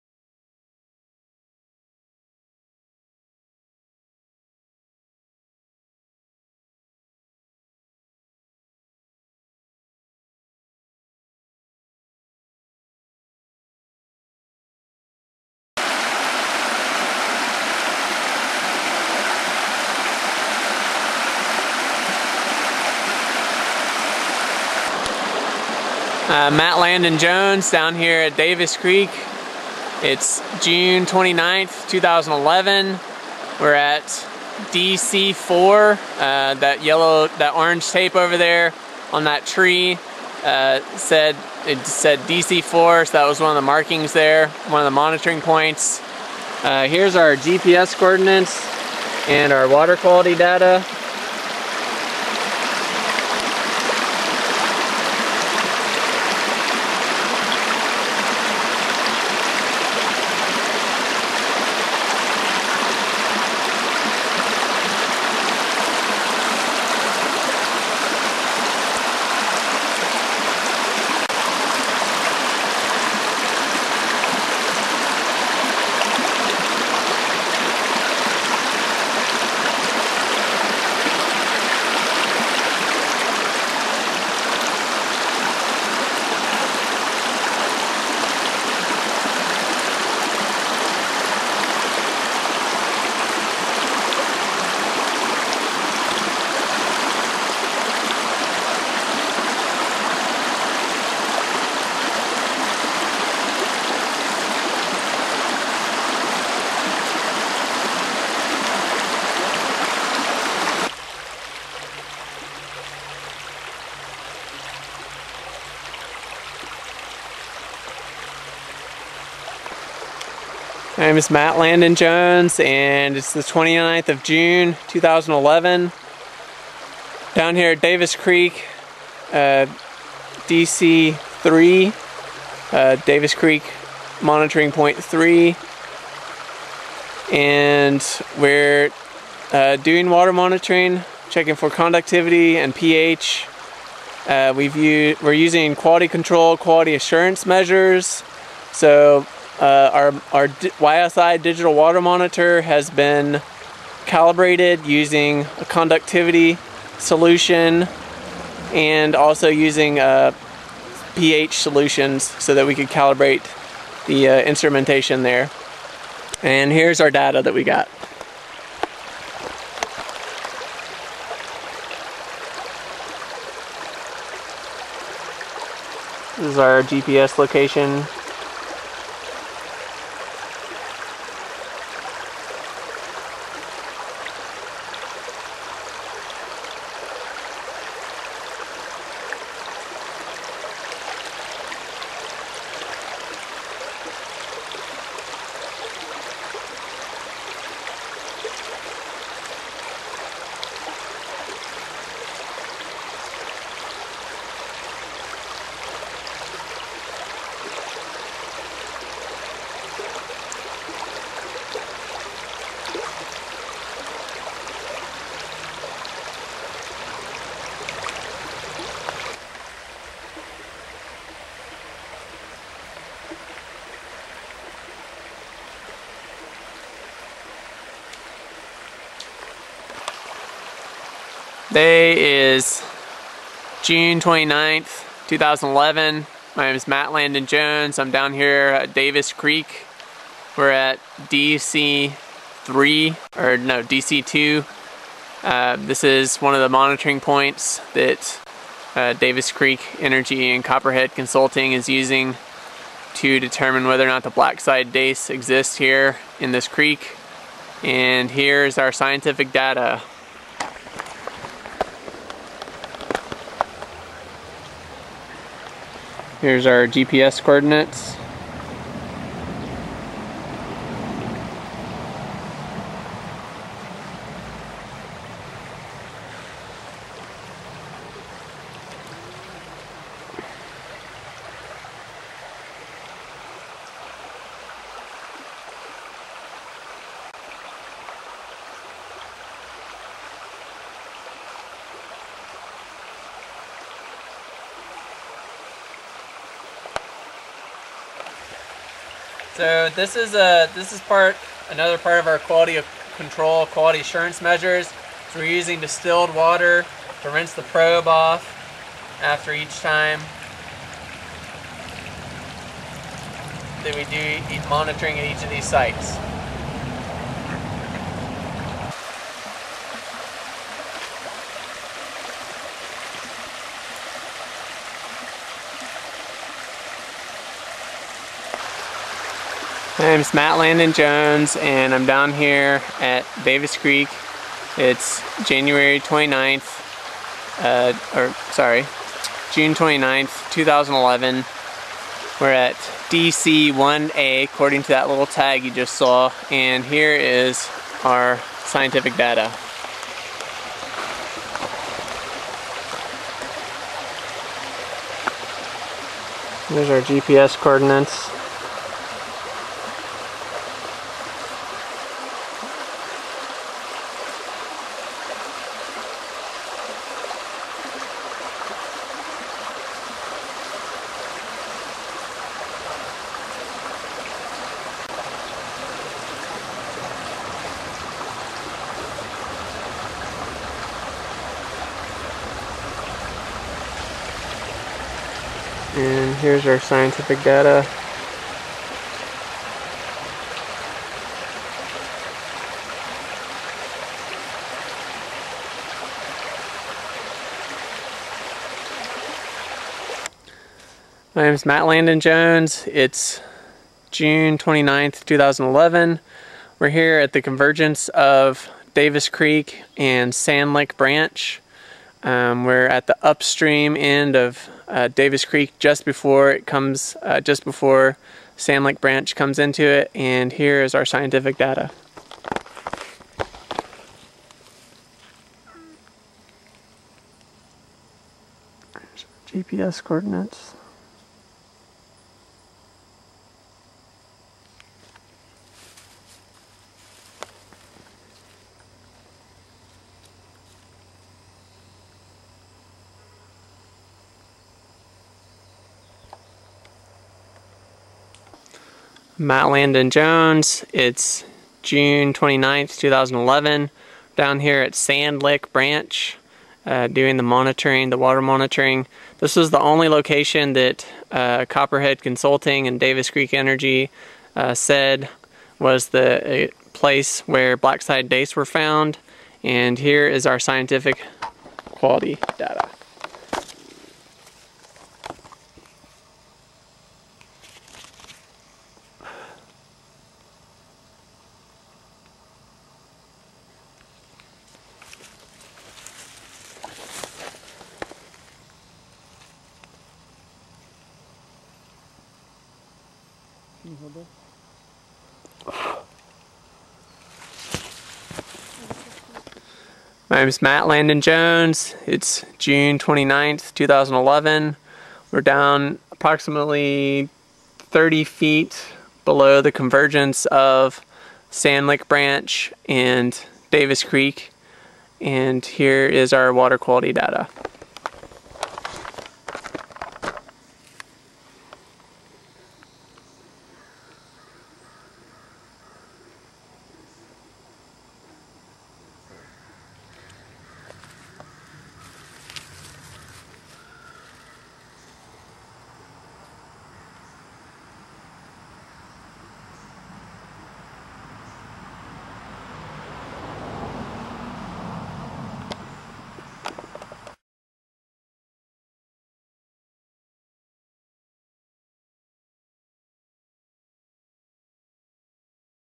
6-29-2011 United Mountain Defense volunteer water monitoring in Davis Creek.wmv
United Mountain Defense volunteer Matt Landon Jones re-visited numerous water monitoring sites along Davis Creek in Campbell County, TN on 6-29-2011.  He used quality control / quality assurance measures before, during, and after the monitoring to make sure that the readings on the YSI digital water monitor were as accurate as possible.  These measures included calibrating the equipment for both conductivity and pH before going out into the field on this day and washing the probes off with distilled water after each sample and video taping the data collection along with GPS coordinates for each monitoring